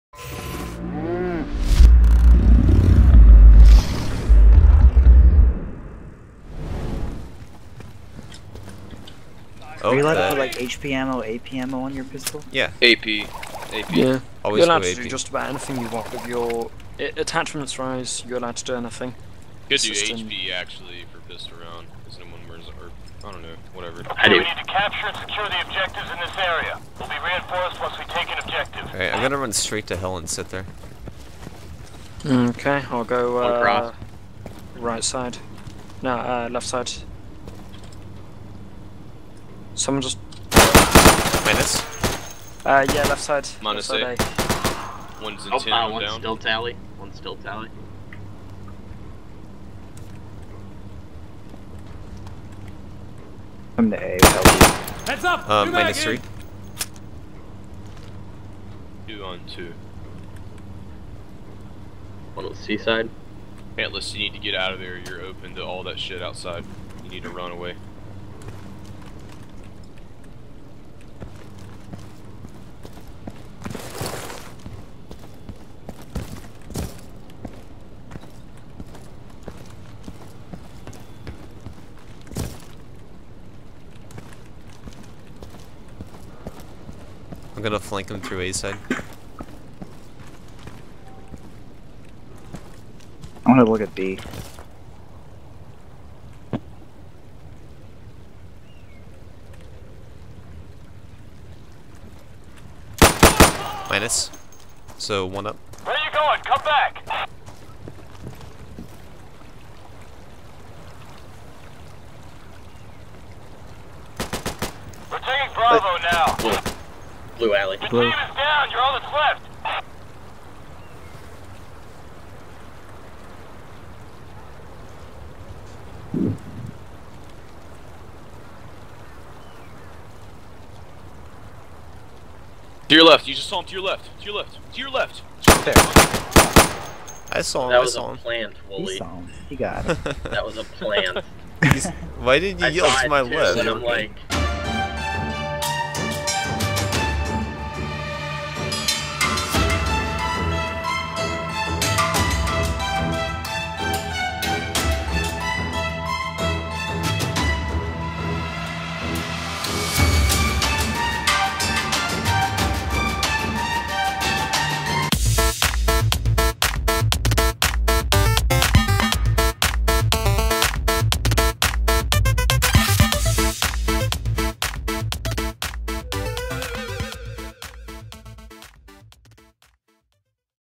That was a plant Wooly
A pistol round to choose sides during a PUG and Wooly made an understandable bad call that i couldn't resist sharing xD